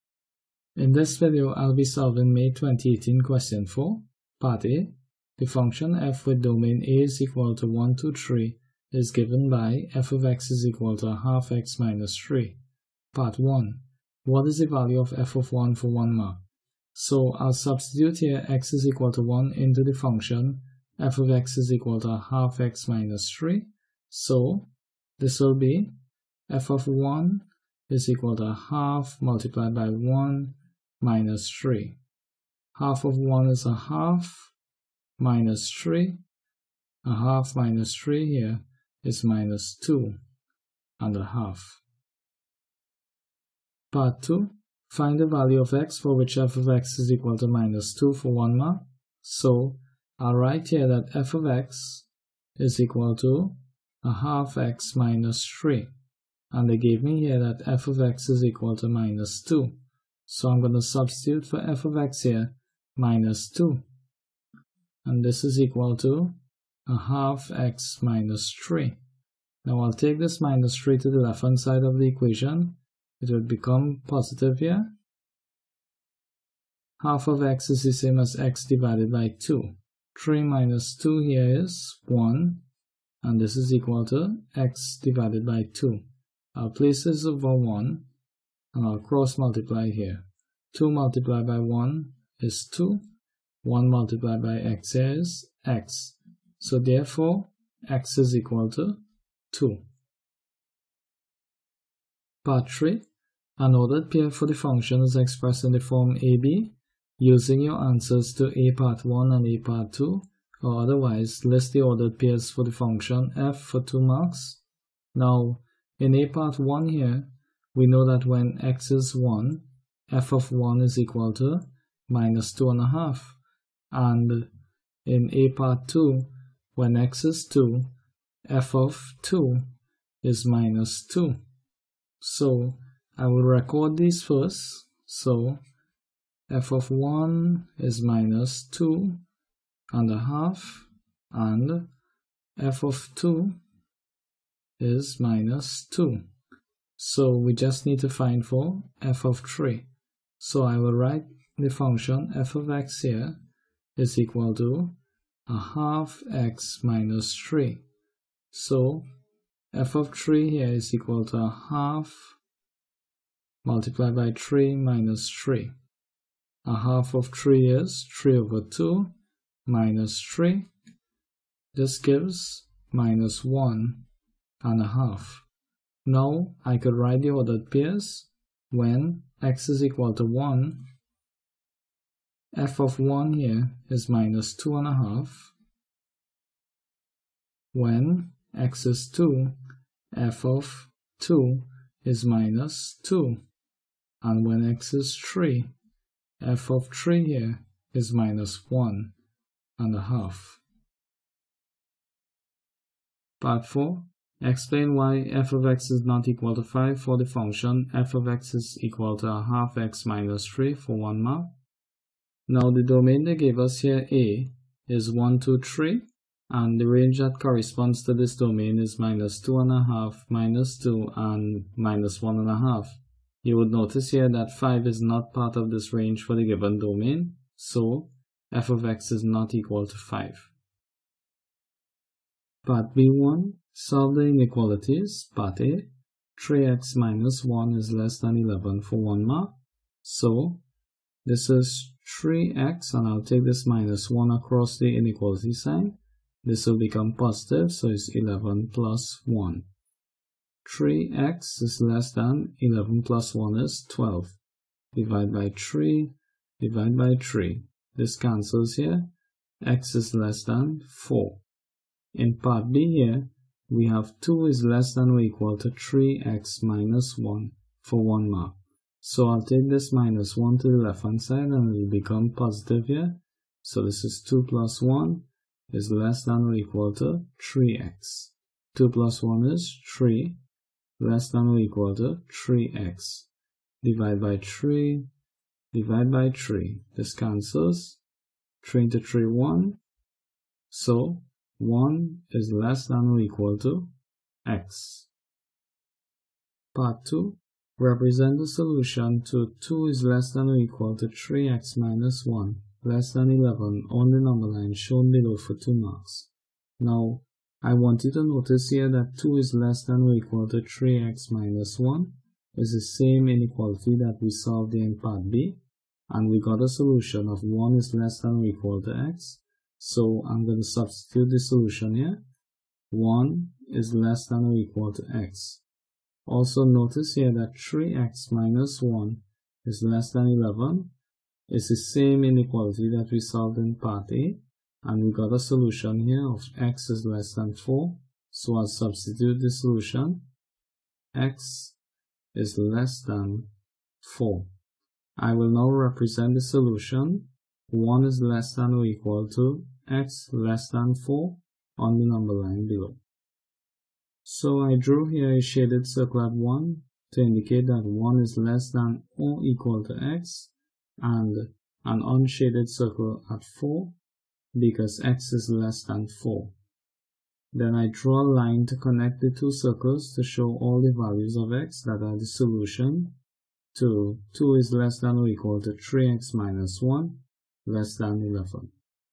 CSEC Mathematics May 2018 Paper 2 Question 4
Dear Students,

a) In order to get the most out of this video, please review the following topics:
1. Functions
2. Linear Inequalities

b) Attempt the question before looking at my solution.

c) Use my solution to make corrections as well as to reinforce mathematics previously learnt.

NB: Cramming past paper solutions without learning the mathematics properly is a sure way to confuse and frustrate yourself very quickly.

Mr. O. J. Khan